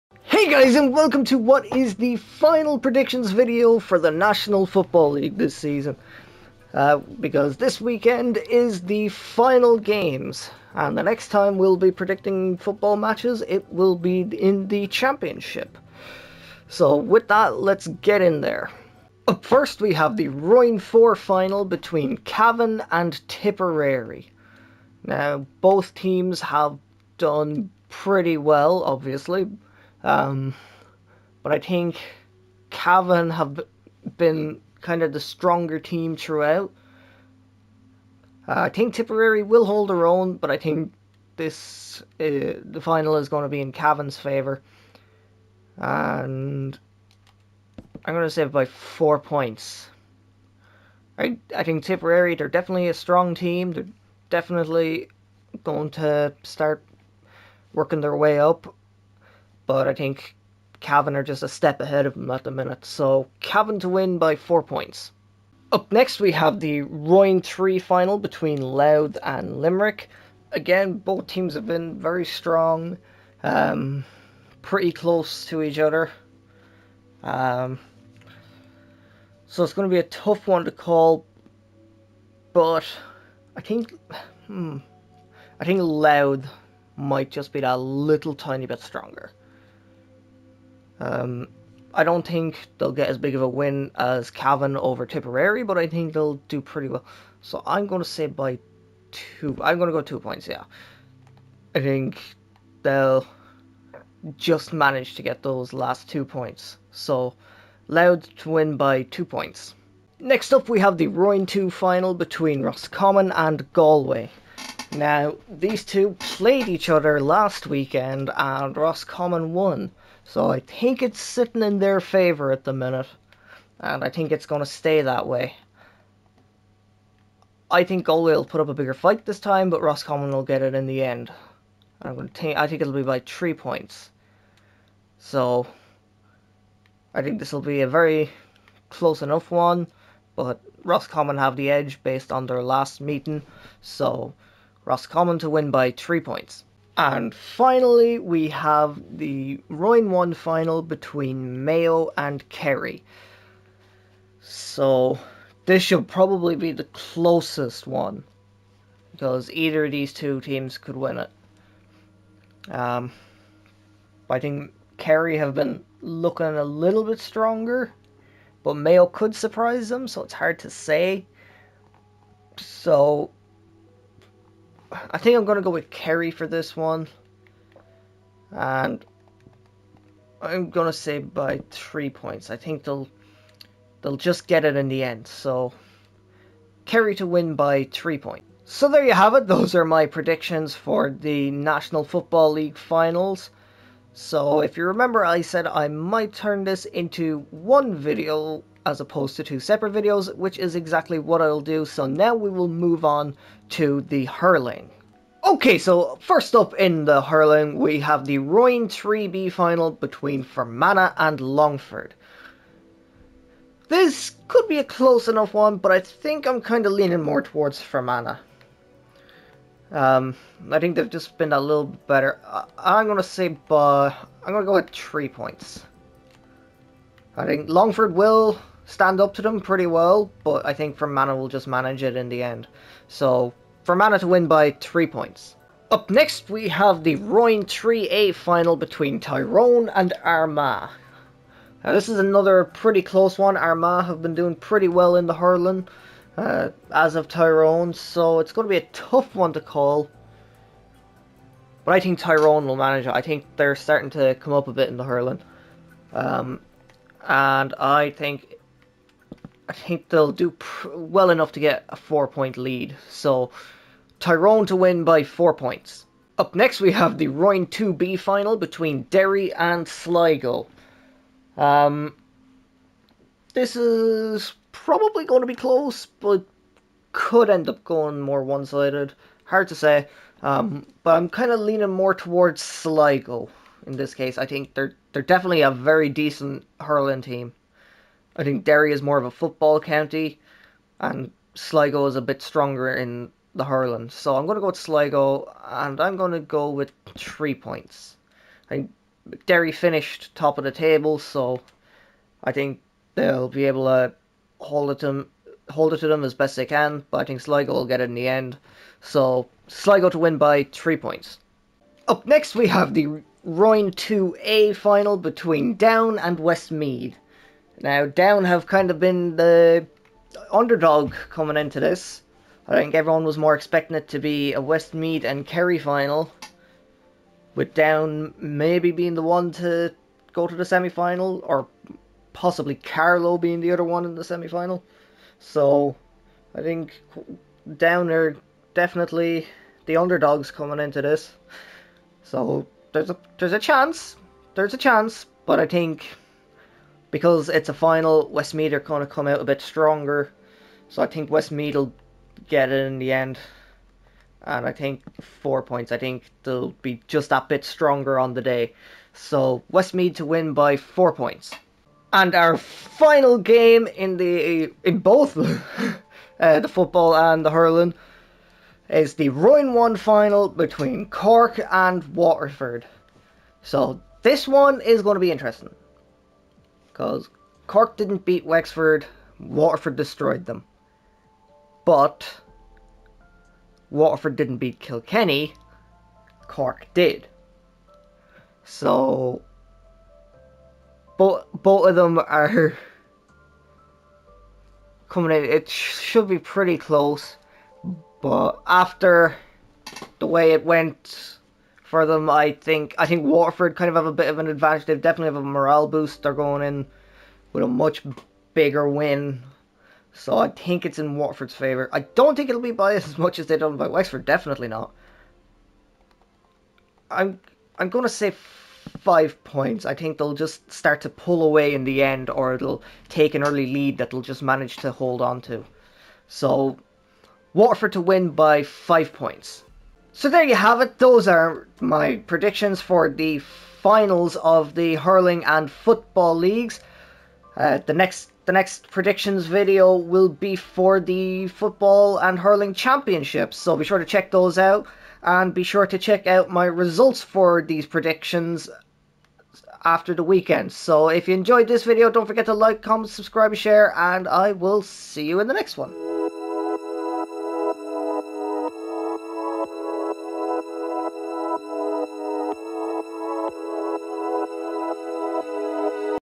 National Football League & National Hurling League Finals Predictions 02/03 April 2022
Here are my predictions for the football and hurling league finals 02/03 April.